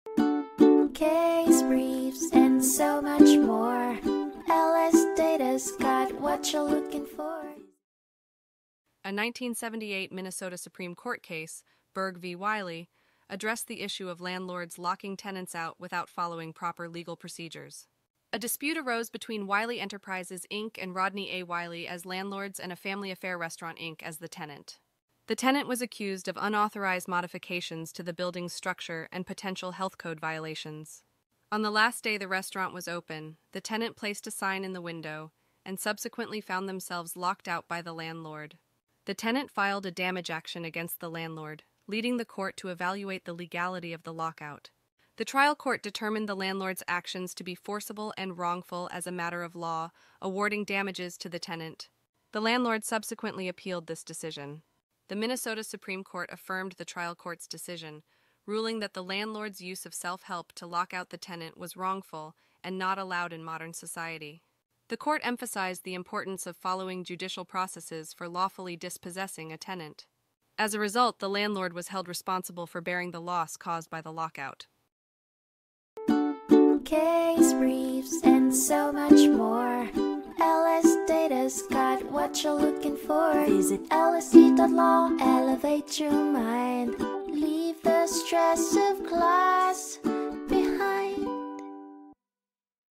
Berg v. Wiley (1978) Overview | LSData Case Brief Video Summary
The case is about a dispute between a landlord and a tenant. The tenant allegedly made unauthorized changes to the building structure and violated health codes, prompting the landlord to attempt to retake possession of the premises. The tenant closed the restaurant on the last day and placed a sign in the window. The landlord changed the locks and locked the tenant out, resulting in a damage action by the tenant. The court considered whether the tenant abandoned or surrendered the leasehold before being locked out and whether the landlord's self-help repossession of the premises by locking out the tenant was rightful.

Berg v. Wiley (1978)
Minnesota Supreme Court
264 N.W.2d 145